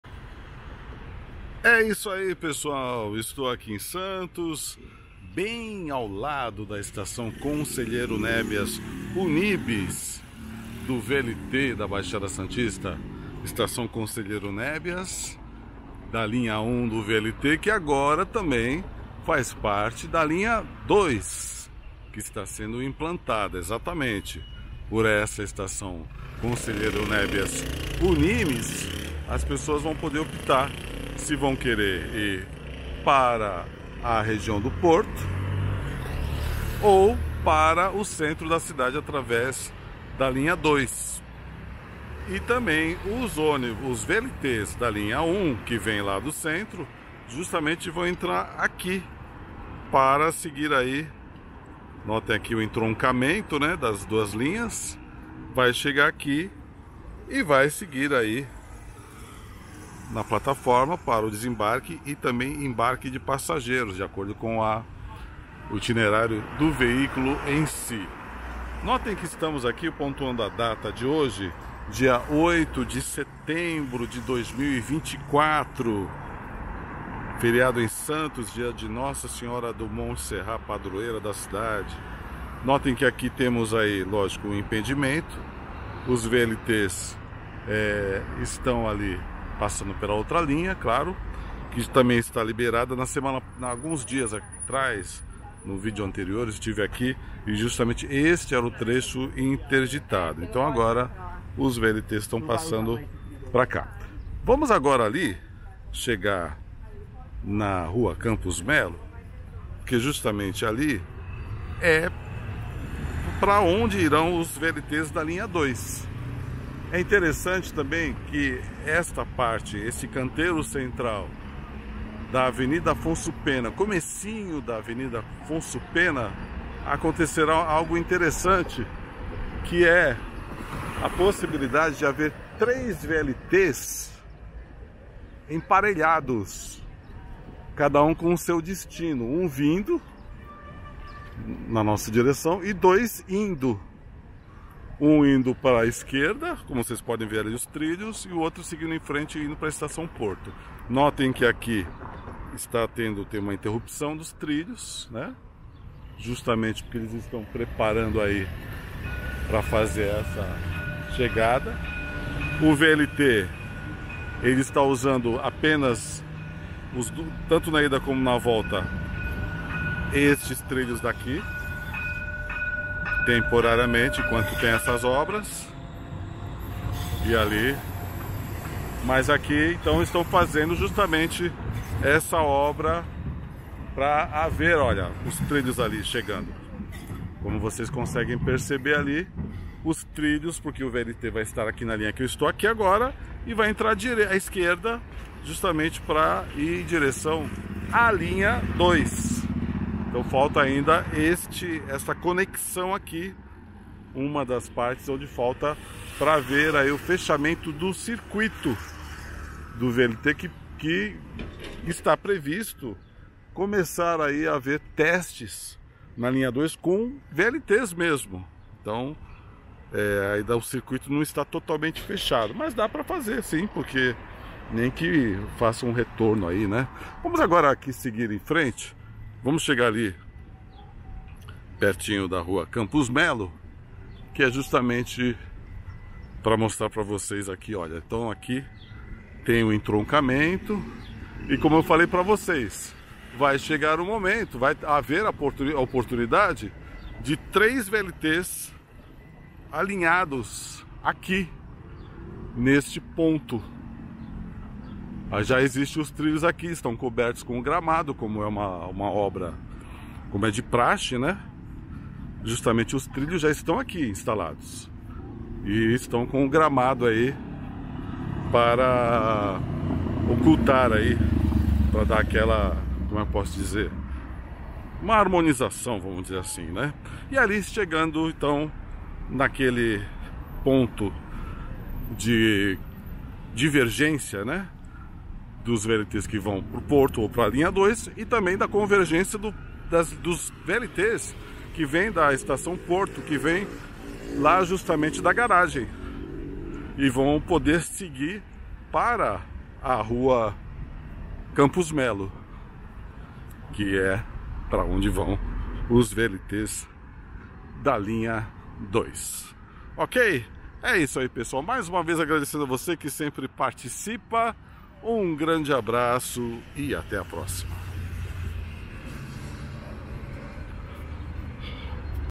Falta pouco para a conexão completa das linhas 1 e 2 do VLT em Santos
Apenas mais alguns metros de trilhos e um aparelho de mudança de via estarão totalmente conectadas a linha 1 com a nova linha 2 do VLT da Baixada Santista, em Santos.

Neste vídeo estamos junto à estação Conselheiro Nébias/ Unimes que atenderá as duas linhas.

Gravado em 8 de setembro de 2024